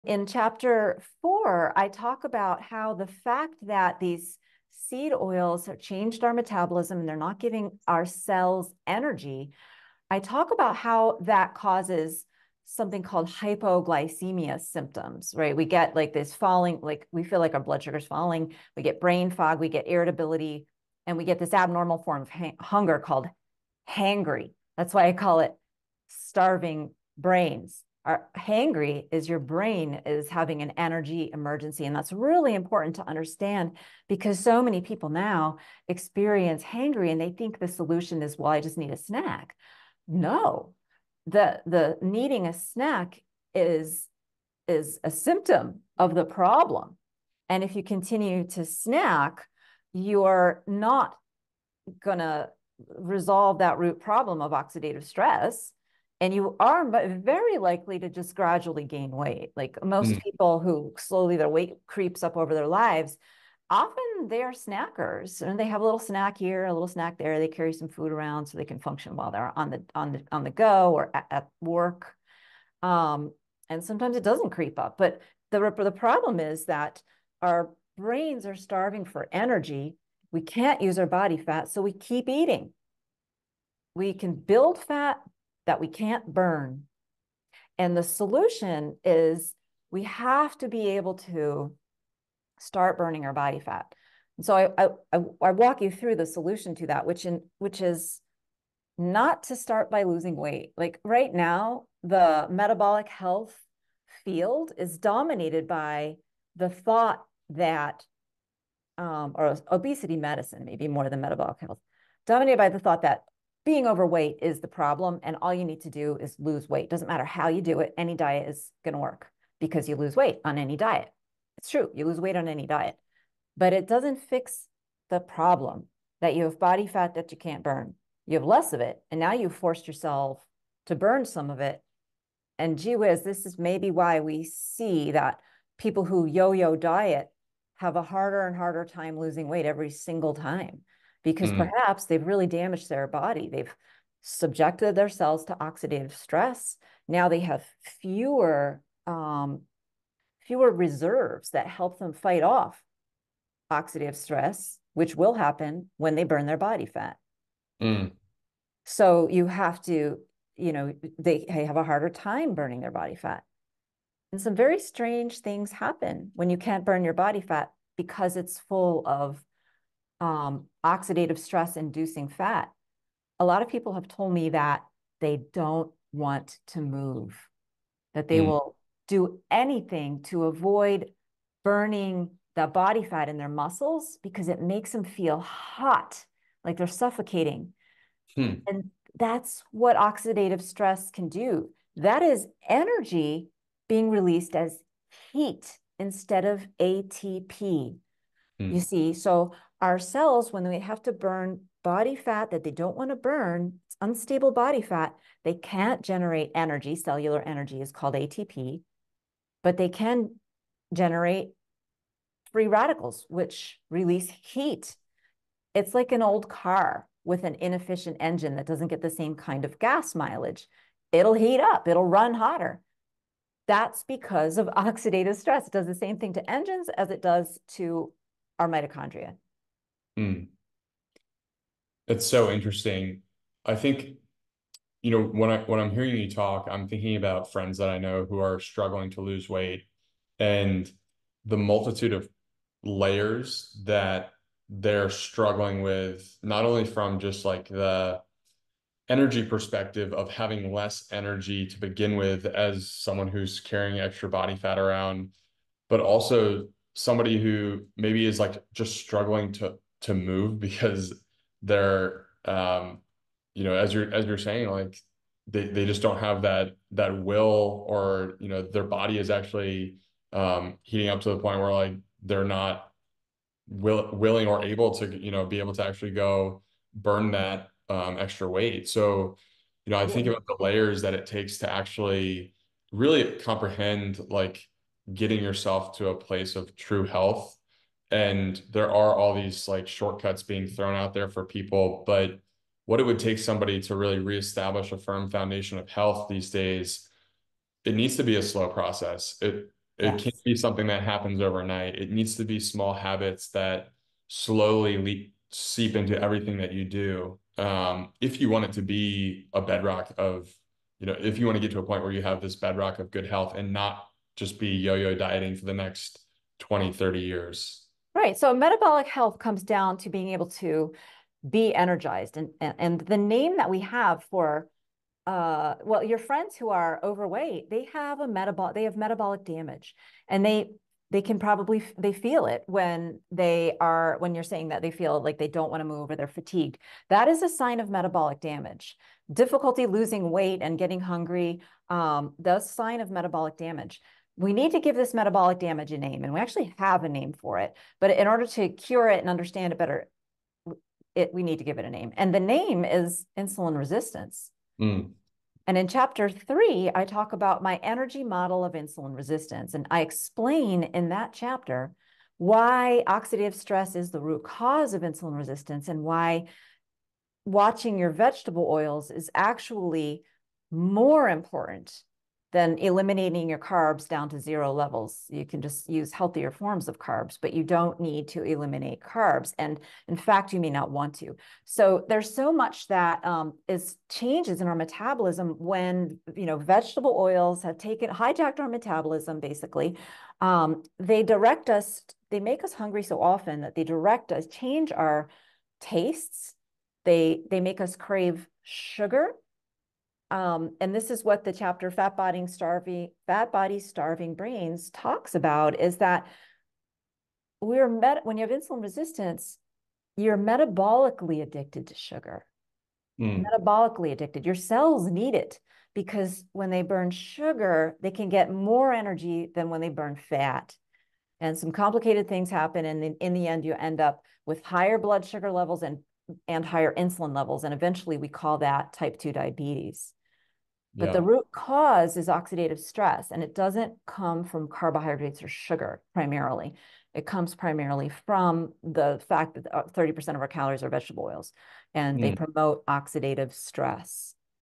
The Unburnable Fat You Didn't Know Existed - Seed Oils & Metabolism - Dr. Cate Shanahan
Dr. Cate Shanahan explains how seed oils disrupt metabolism, causing symptoms like hypoglycemia, brain fog, and increased hunger. It highlights the inability to efficiently burn body fat due to oxidative stress, leading to weight gain and metabolic issues.

She argues that addressing the root cause—seed oils—rather than just losing weight is essential for lasting health improvements.

The hateful 8 seed oils Dr. Cate refers to are:

Canola oil (rapeseed oil)
Corn oil
Cottonseed oil
Soybean oil
Sunflower oil
Safflower oil
Grapeseed oil
Rice bran oil